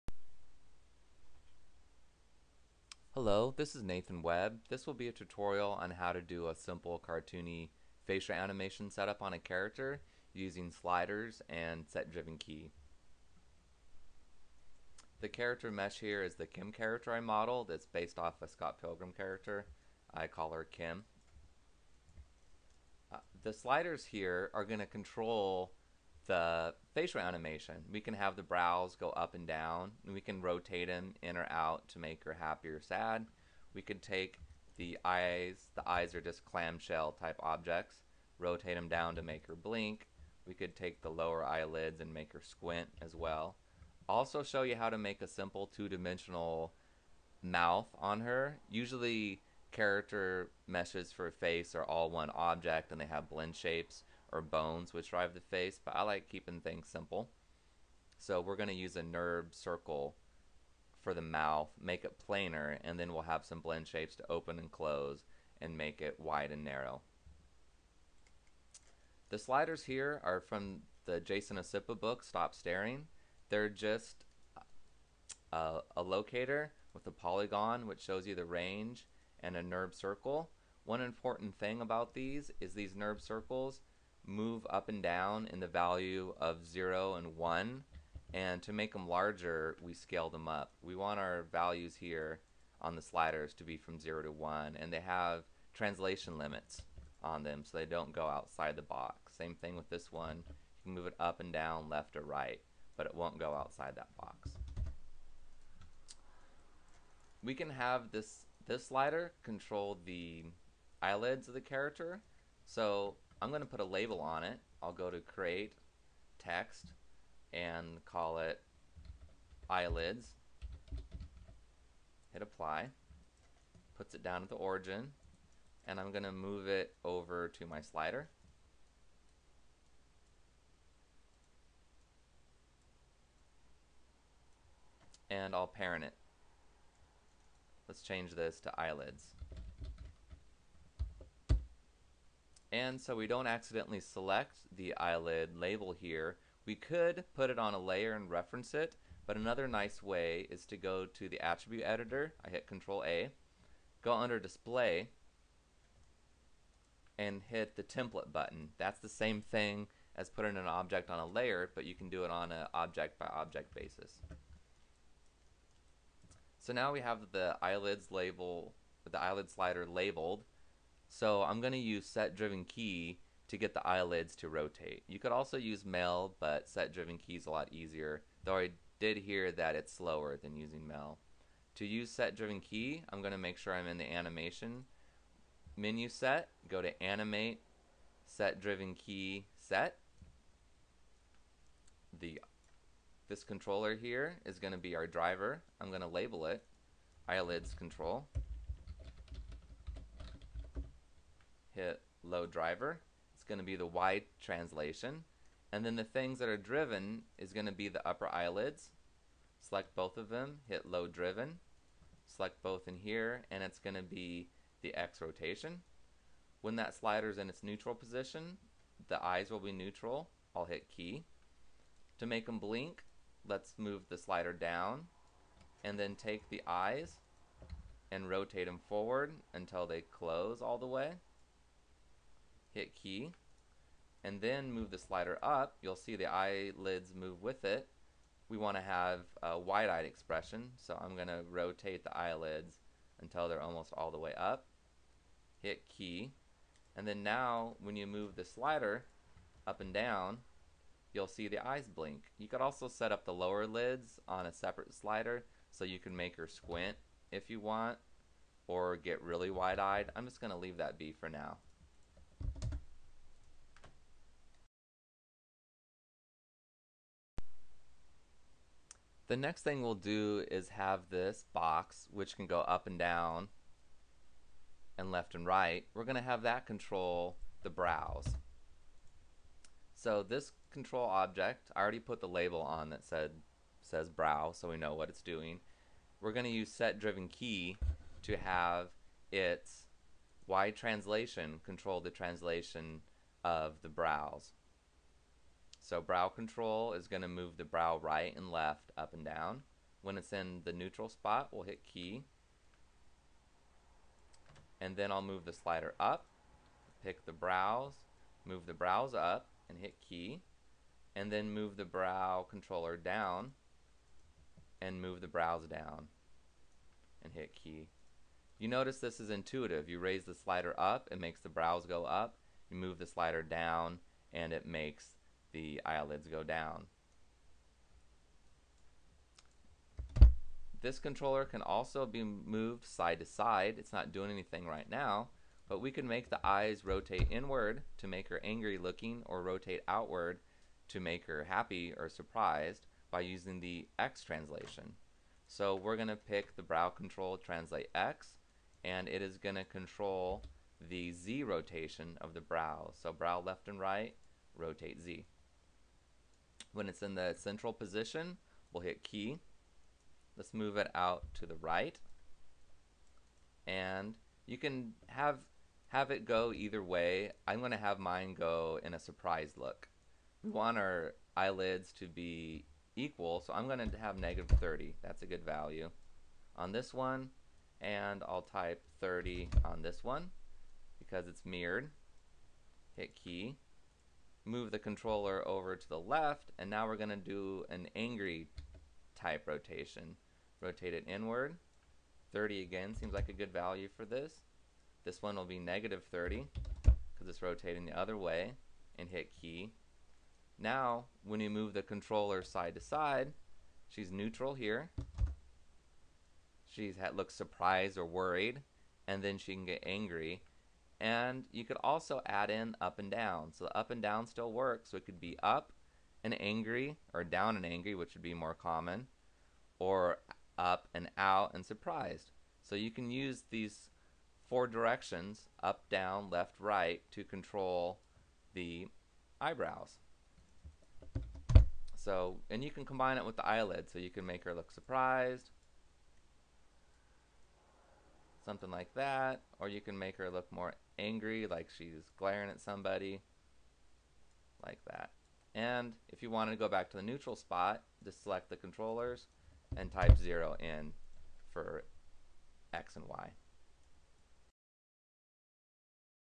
Facial Animation Cartoony Maya Rigging Tutorial Part 1 of 2
How to rig a cartoon character's face in Maya using on-screen facial sliders and set driven key to drive simple facial geometry.  Part 1 covers using an up and down slider to make the character's upper eyelids blink, and using a 4 way slider to get the brows to go up and down, plus rotate inward and outward.  Part 2 shows how to make a nurbs curve mouth, an have a 4 way slider make it open, close, and go wide and narrow by using blend shapes.